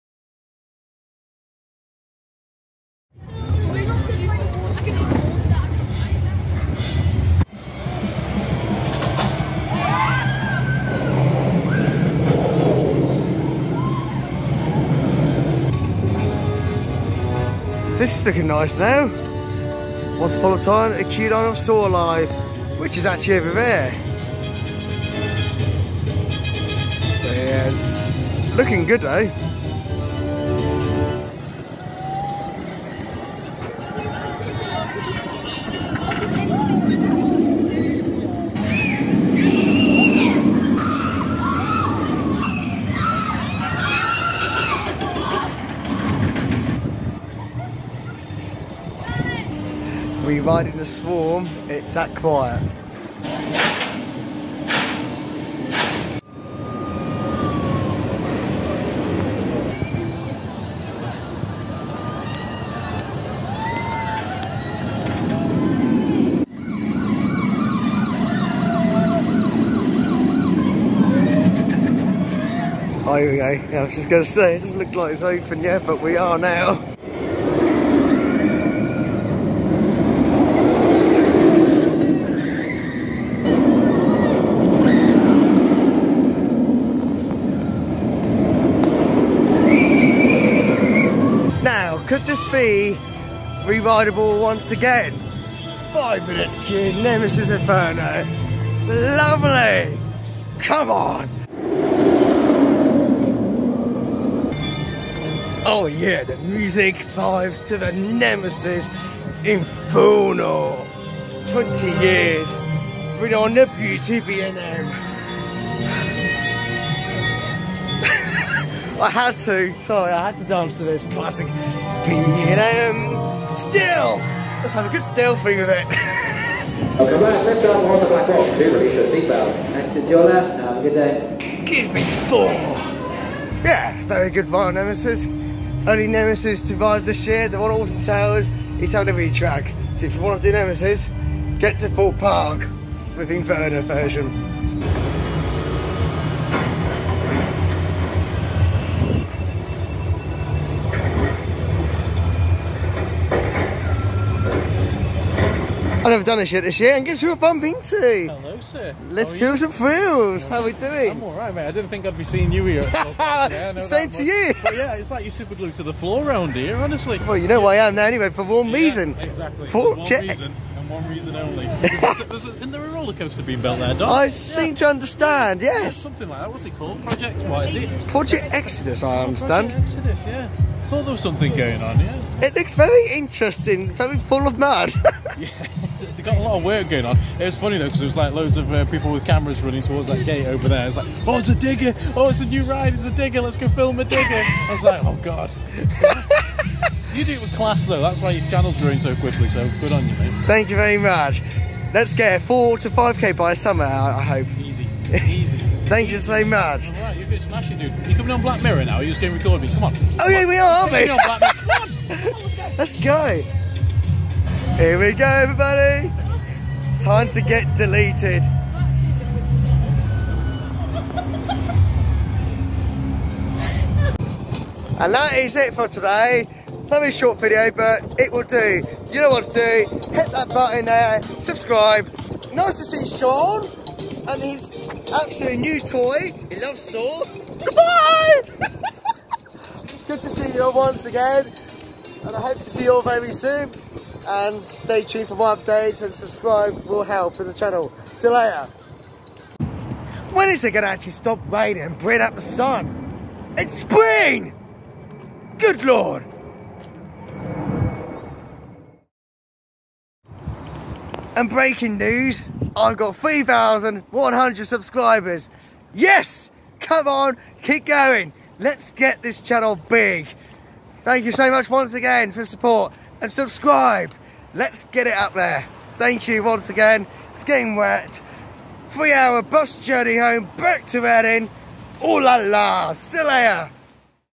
THORPE PARK Resort | A WET BRITISH TUESDAY VLOG 2023!
I arrived at 1pm and I wanted to enjoy the thrills of my life in my own little world, I did a vlog yesterday and it was SUPER DRY AND SUNNY!!! However it was rather wet day and also a very long day to get to the resort by buses from Reading within 4 hours due to bad traffic and hopefully it'll be a easy journey for next time.

Filmed - 28/03/23 featured... @LiftHillsandThrills and Coaster Dan!

MERCHANDISES!!!